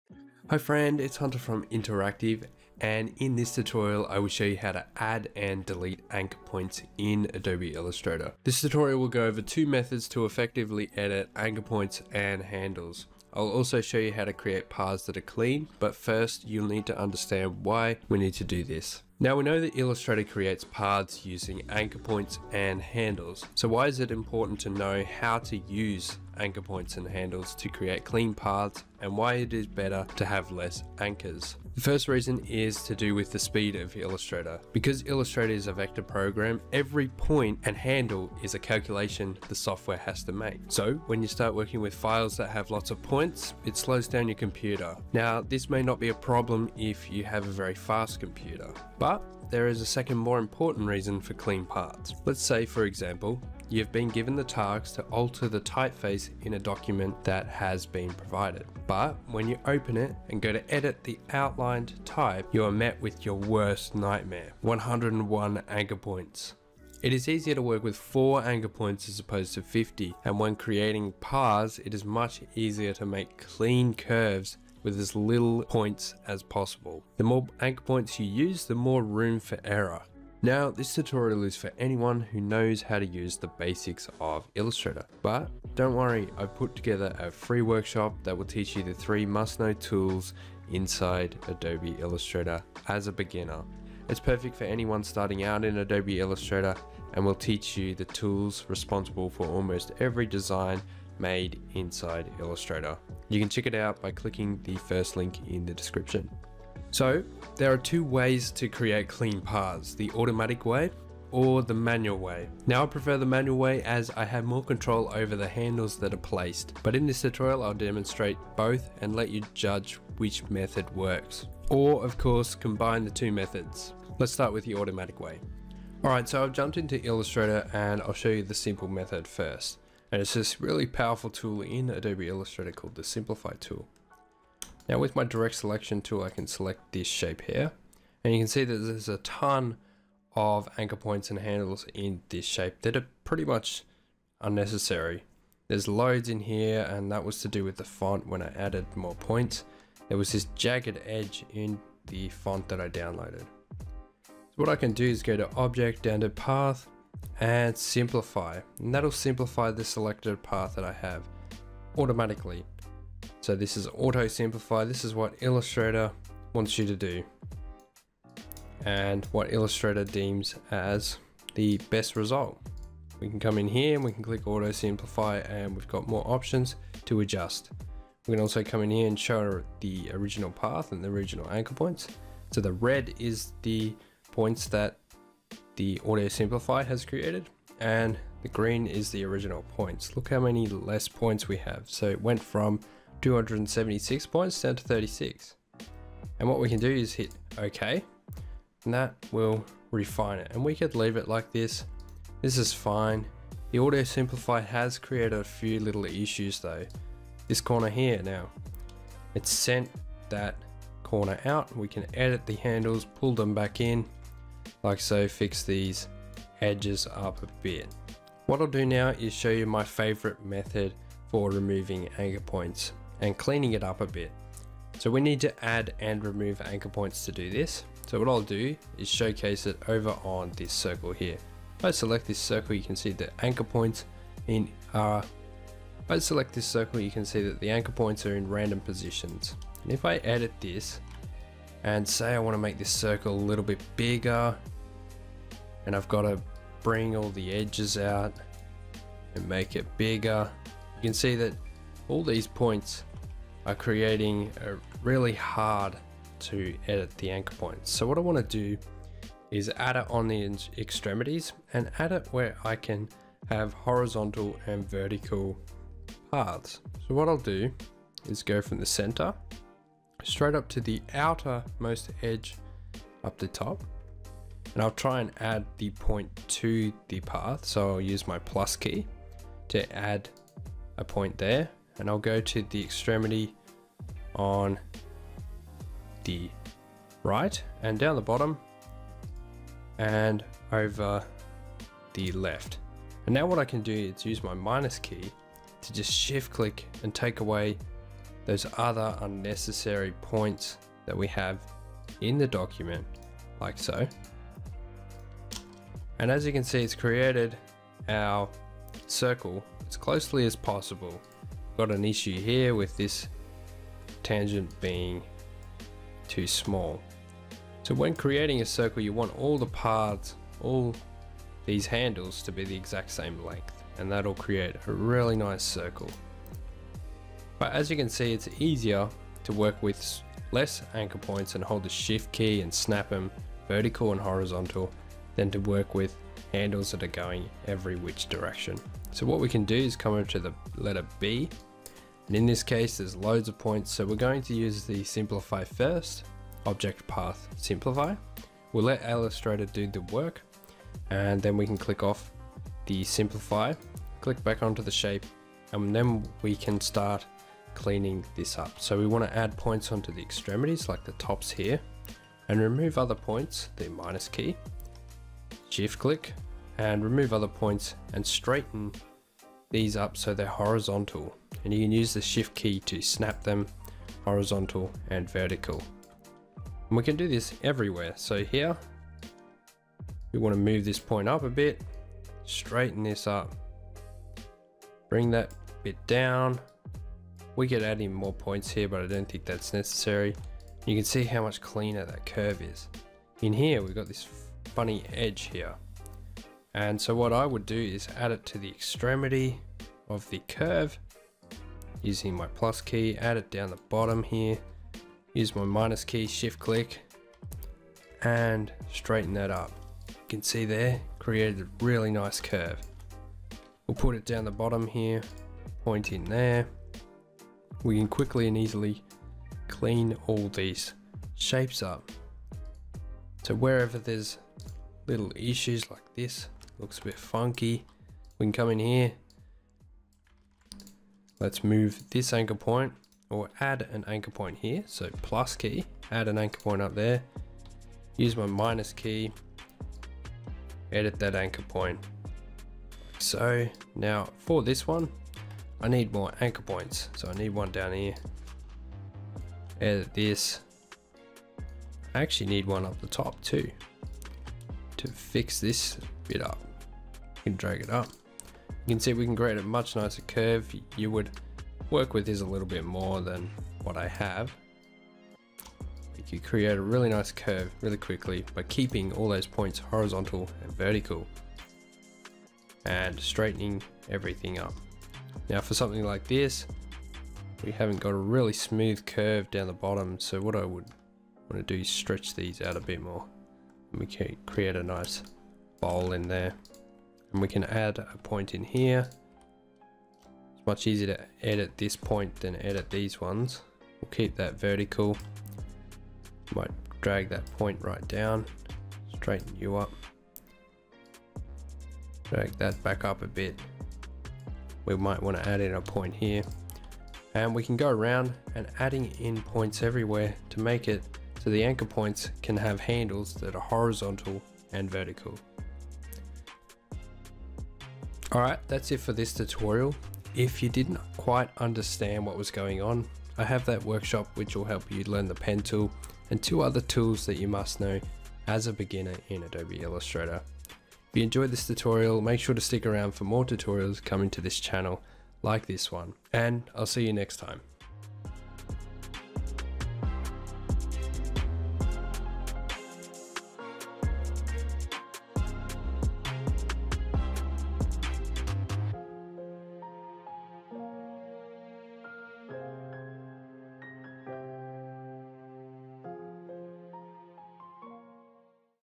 Create Clean Paths in Adobe Illustrator | How to Add and Delete Anchor Points
Creating clean paths inside Adobe Illustrator is good, especially if you are working in a team. Join my free workshop here to learn more about Adobe Illustrator

Some of the artwork was sourced from Envato Elements.